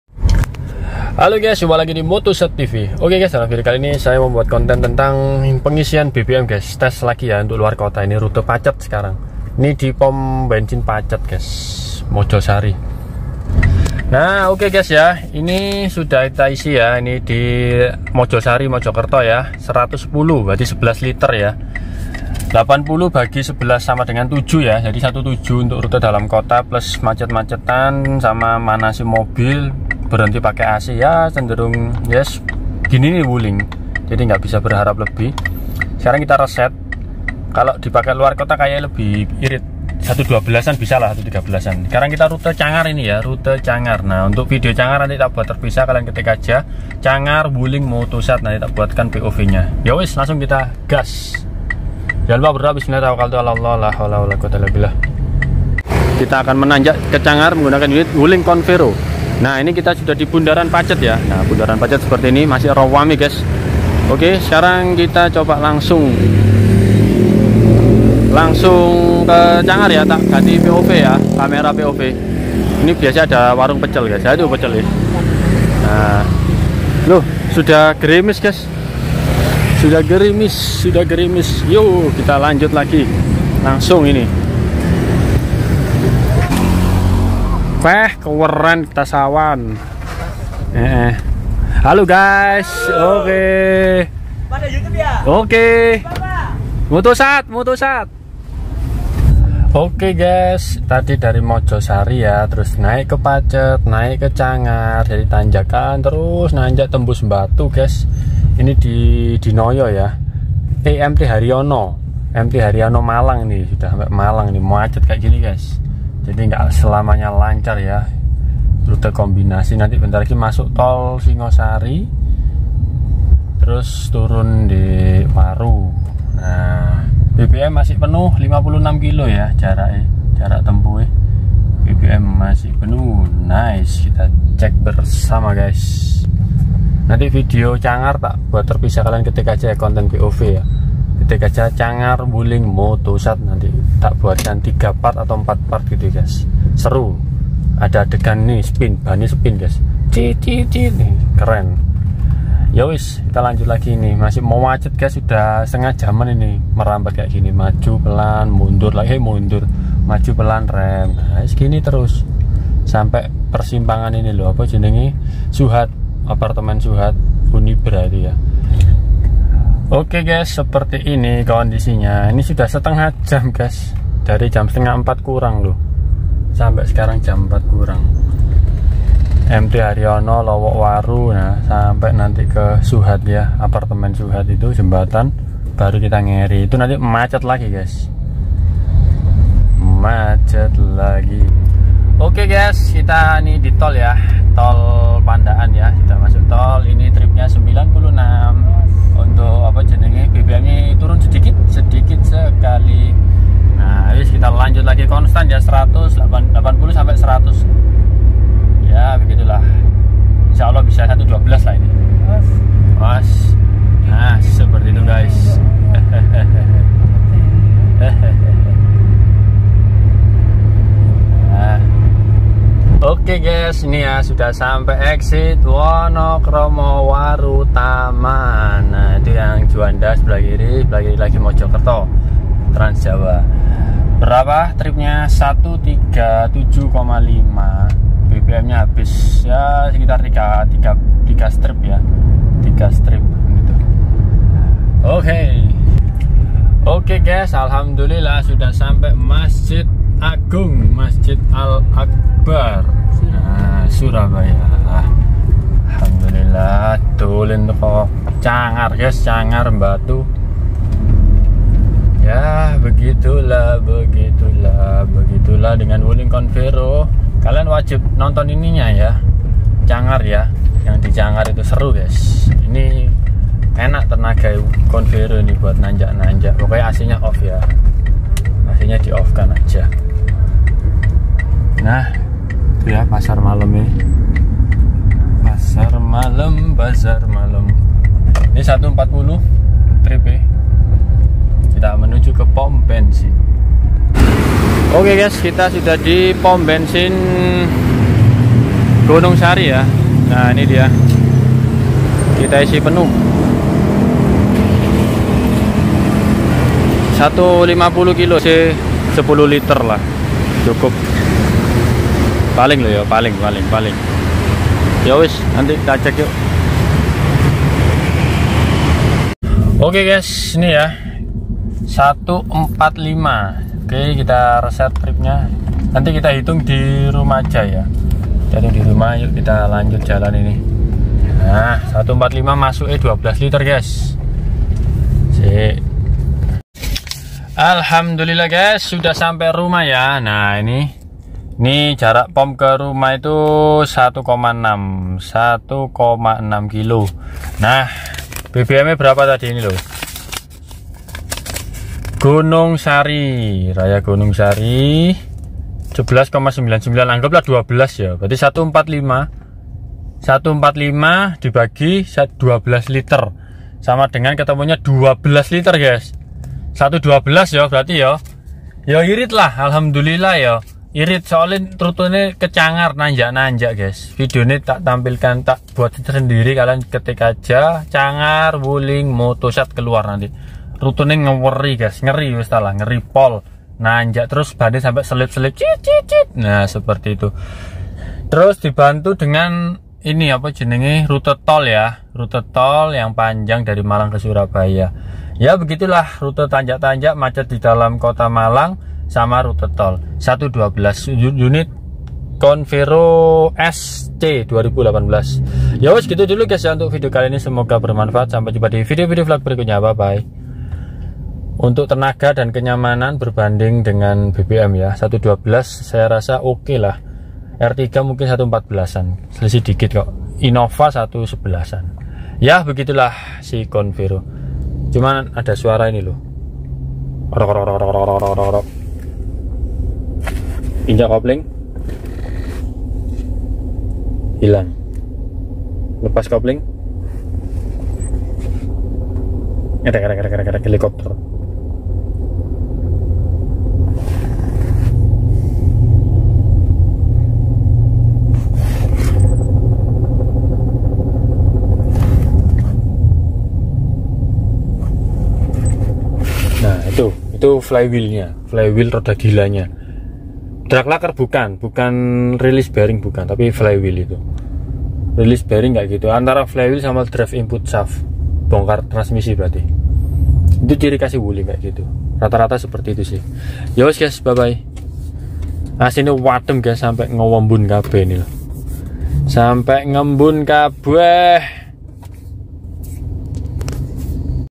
Tes BBM Wuling Confero S 2018, Lumayan Irit - Rute Luarkota, Tanjakan, Macet, Jalan Tol
Mobil     : Wuling Confero S 1.5C M/T 2018
Kamera : Gopro Hero 7 Black dan HP Samsung A54 (1440p / 60fps)
Lokasi    : Surabaya

Terimakasih